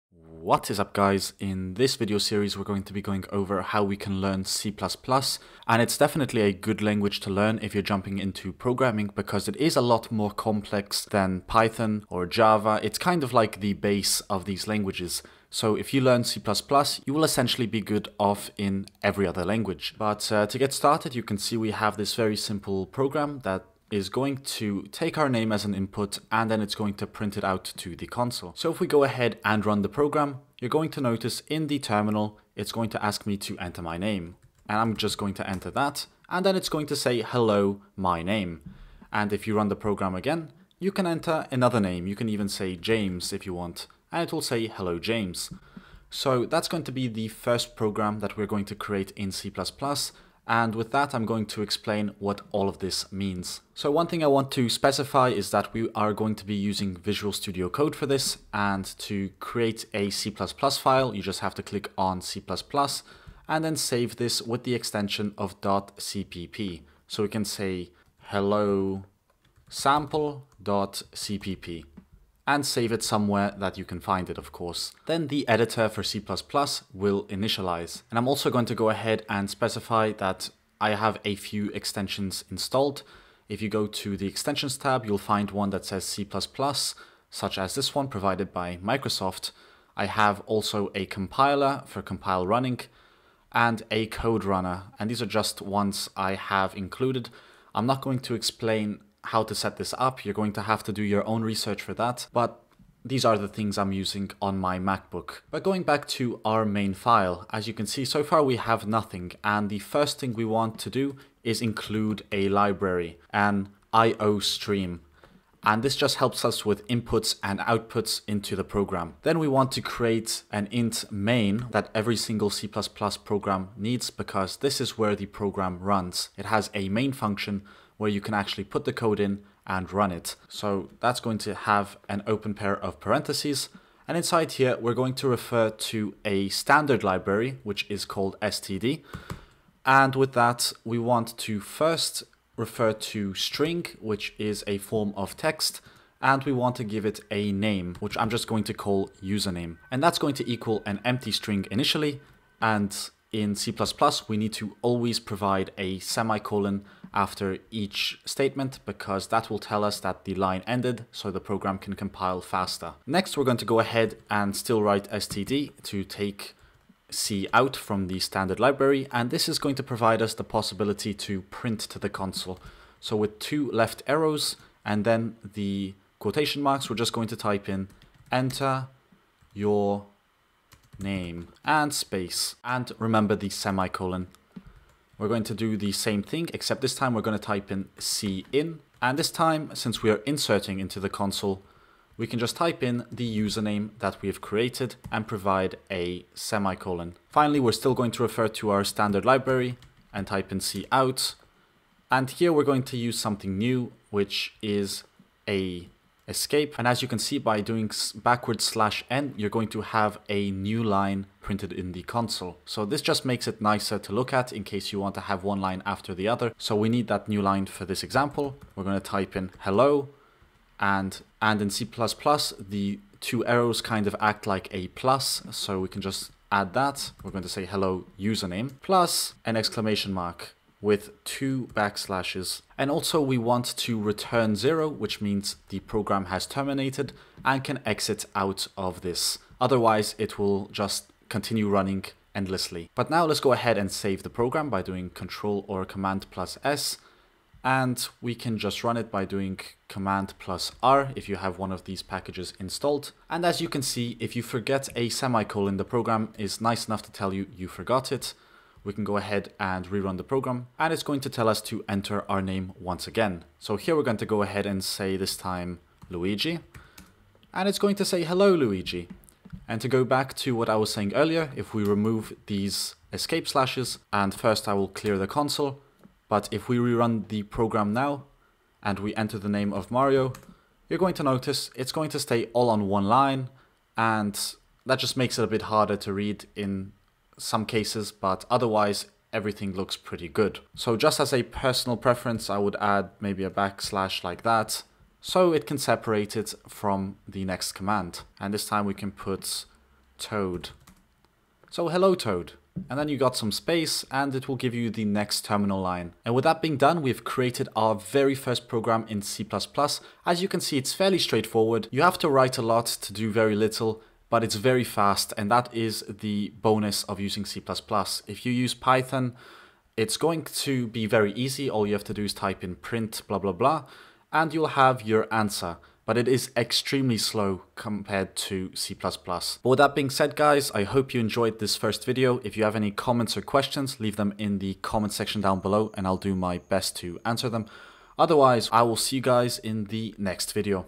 Learn C++ from Scratch Tutorial 2021 (Lesson 1: Hello World!)
In this series we're going to be learning C++, a great language to learn if you're just starting to learn about code. It will be a steep learning curve, but after this, the rest of your life will be easy, that's for sure.